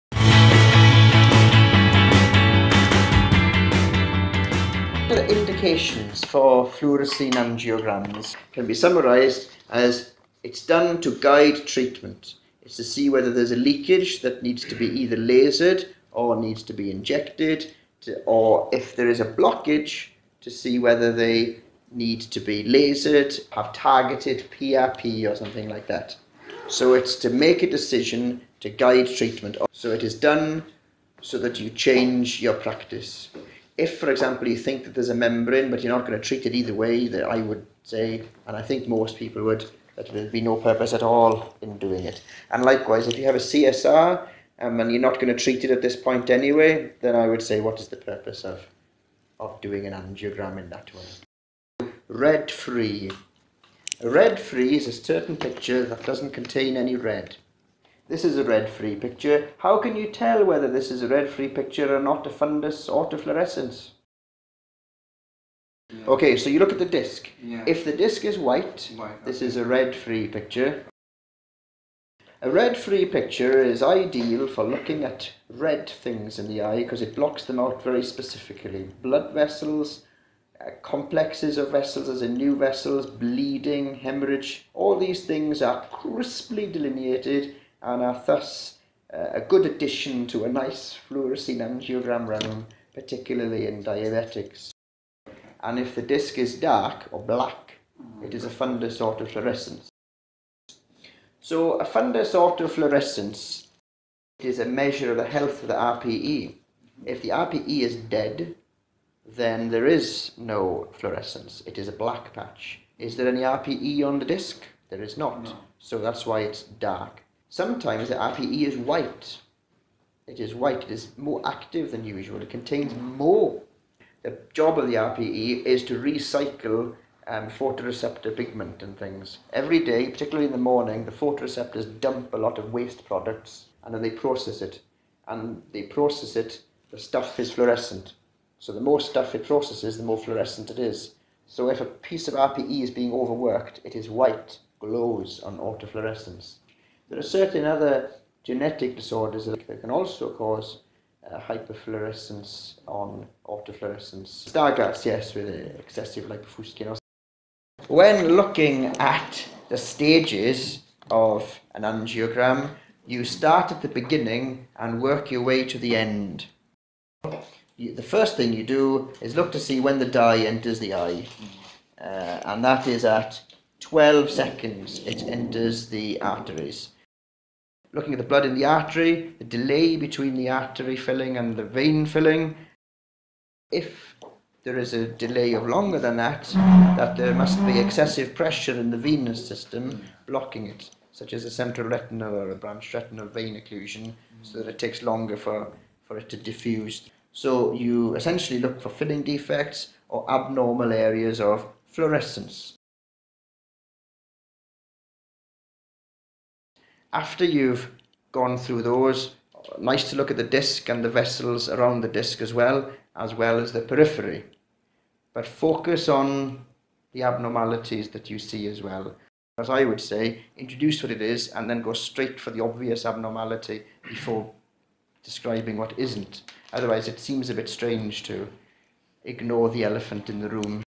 FFA tutorial - Part 2
Fluorescein angiography, FFA - an introduction about the basic principles of the test. An FFA teaching session by Mr Williams Medical Retina consultant